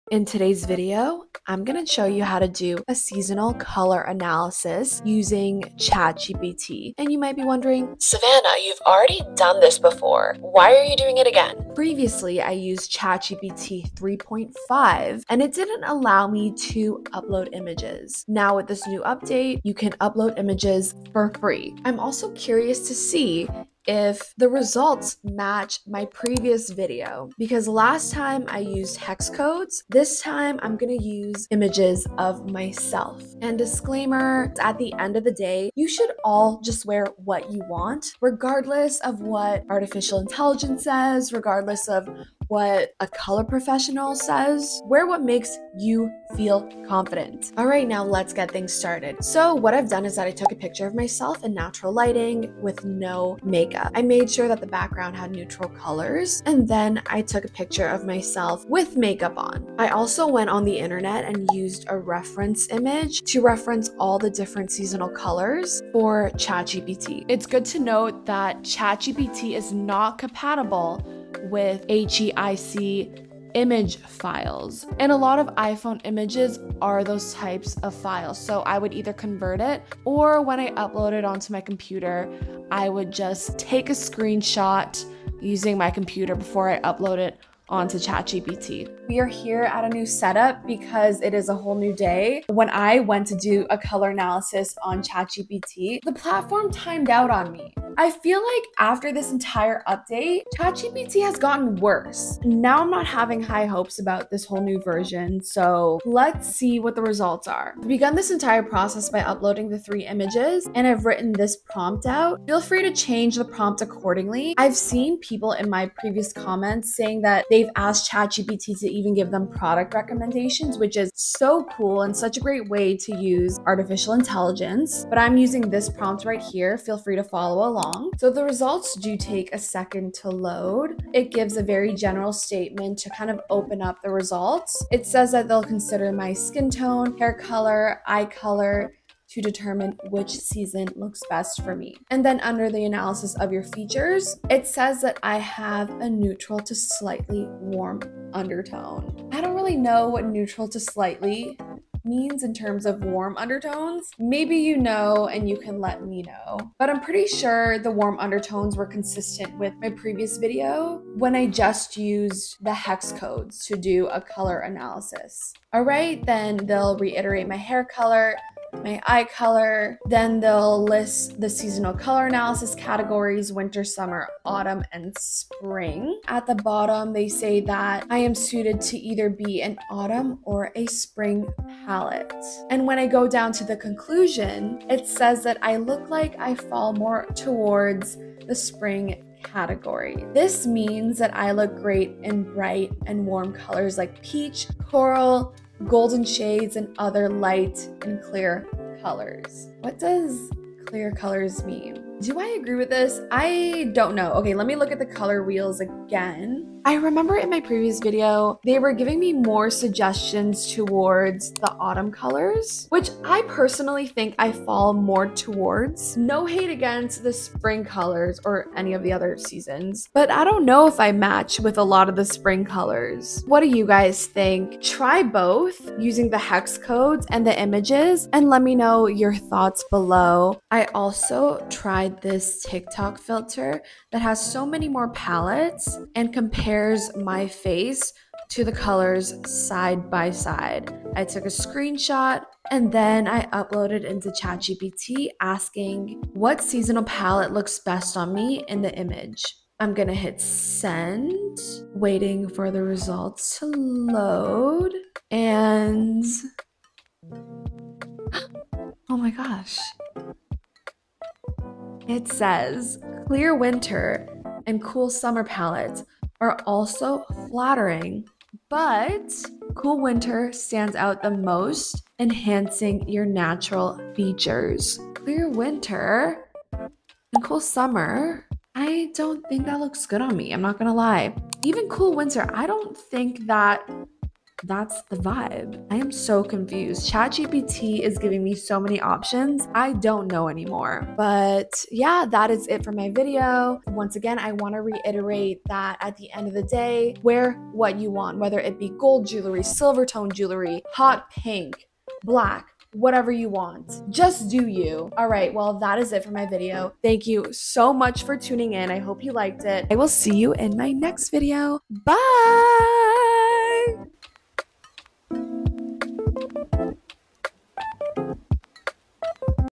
🎨 UPDATED: How to do a *FREE* COLOR ANALYSIS using ChatGPT: Uploading Images
— TIME STAMP —
Explanation & ChatGPT Updates - 0:00
Step #1 - 0:47
Step #2 - 1:28
RESULTS - 2:12
Using TikTok to do a Color Analysis - 3:50
Outro - 5:05